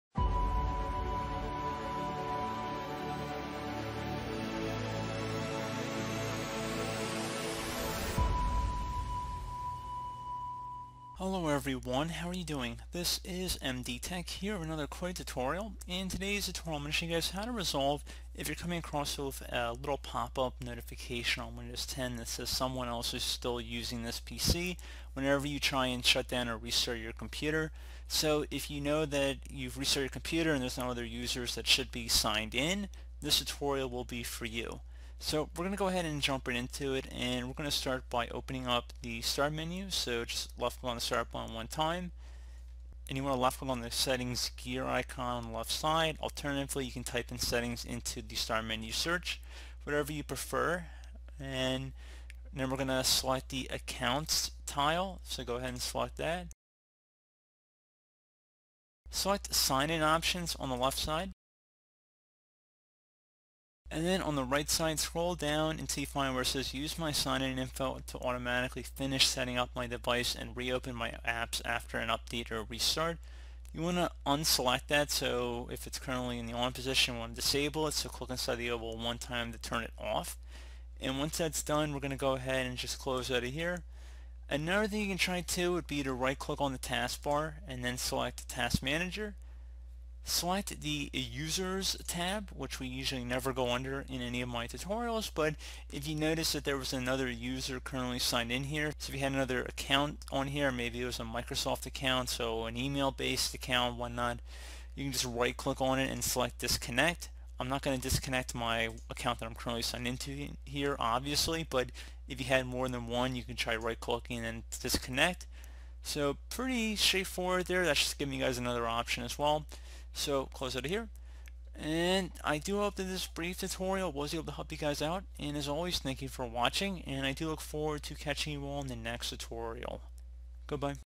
How to Fix ‘Someone Else Is Using This PC’ Error in Windows 10 [Tutorial]
When you ask your PC to stop or restart, Windows first checks if other Windows user accounts have active sessions. This happens if you lock your Windows session and log in with another user account before logging out.

So Windows 10 won't load. Or maybe it starts up, but crashes a lot. Either way, you need to fix the problem before you can use your laptop. If your screen is rotated, we have an easy fix for that. If the operating system is booting slower than molasses, you can also use power settings to speed up its launch. Otherwise, here are a few ways to repair Windows 10.

Several Windows users have been reaching us with questions after encountering the “Someone else is still using this PC” error whenever they try to Shut down or Restart their computer. Most affected users are reporting that the issue occurs even if they try to log in with a different user account. As it turns out, the issue is not exclusive to Windows 10 as it’s also encountered on Windows 8.1.

What’s causing the ‘Someone else is still using this PC’ error?
We investigated this particular issue by looking at various user reports and trying out different repair strategies that other affected users have recommended for this particular issue. As it turns out, several different scenarios will trigger this particular pop-up. Here’s a shortlist of potential culprits that might be responsible for the ‘Someone else is still using this PC’ error:

The issue is caused by a Sign-in Option – As it turns out, this particular issue mostly occurs due to a change inside the Sign-in Options menu that forces the machine to use the sign-in info to automatically finish setting up the device and reopen apps. If this scenario is applicable, you can resolve the issue by turning off one option inside the Sign-in Option.

The previous user is still connected – Another possibility is that the previous user that used this PC hasn’t completed the logoff procedure. This can happen due to a machine interruption or due to user preference. To fix this issue in this case, you’ll need to use Task Manager to disconnect the previous user.

Windows updates are installing the background – If you’re seeing the issue on Windows 10, chances are your OS is mistakenly thinking a different user is connected, when in fact the WU (Windows Component) is installing one or more upgrades in the background. In this case, you can avoid the error message by finishing to install the updates first.